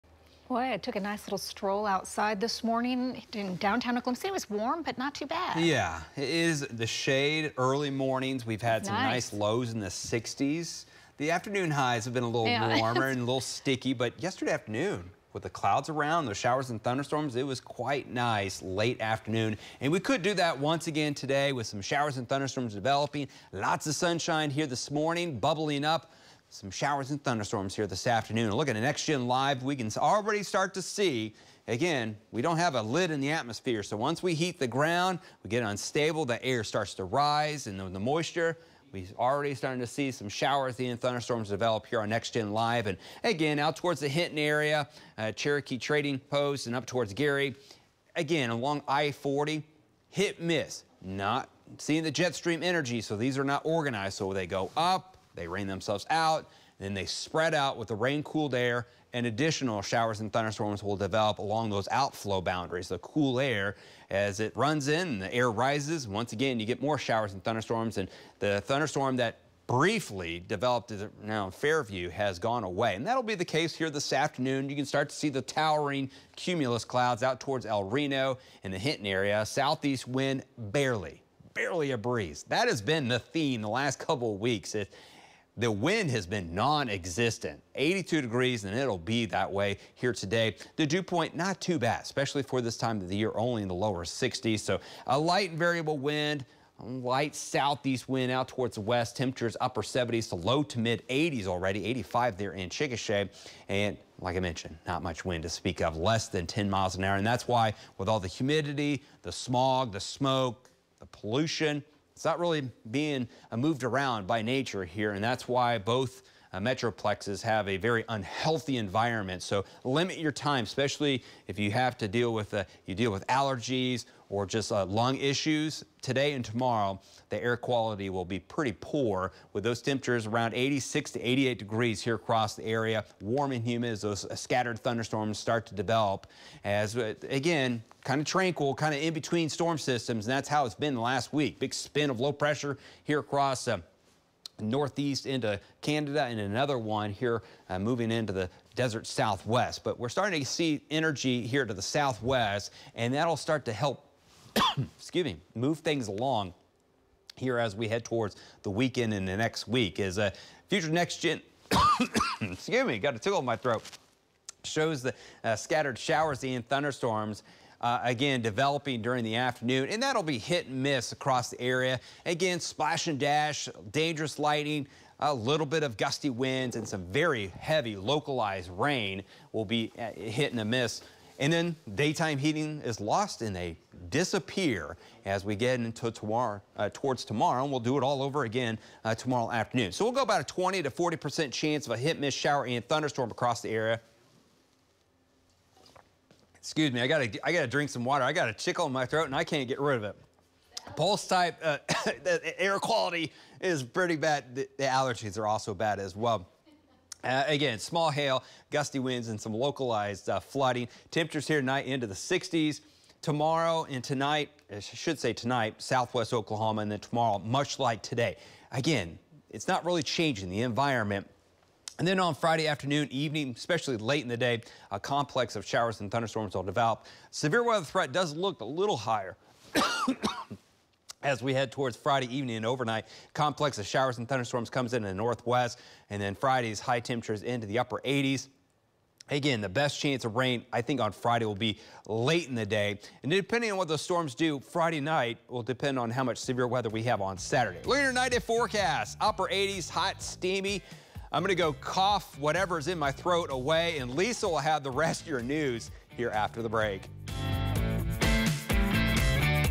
Justin Rudicel's Wednesday Forecast
Meteorologist Justin Rudicel with the Wednesday forecast from the Bob Mills Weather Center.